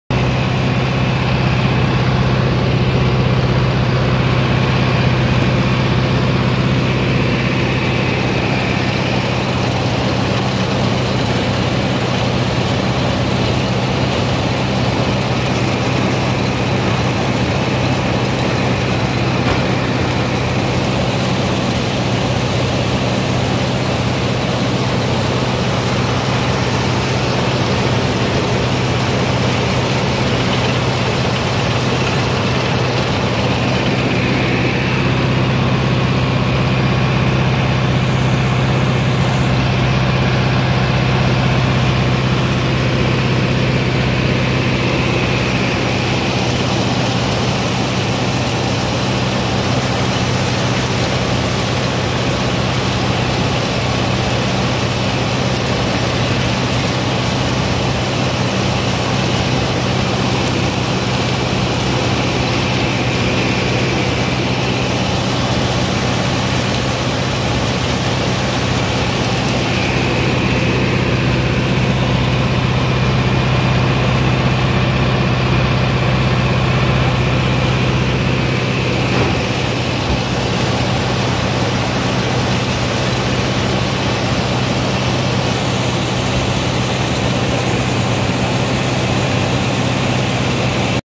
Shelling Corn 2020 — New Idea Two Row Corn Picker with Sheller
Opening up this field and cleaning up the head lands.